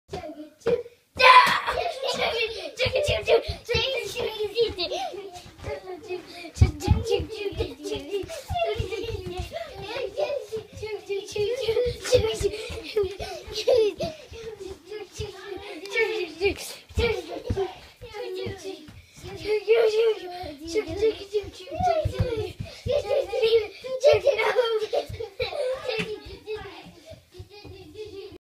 Choo choo goes the pajama train
Beck and Mia preparing for bedtime
August 2014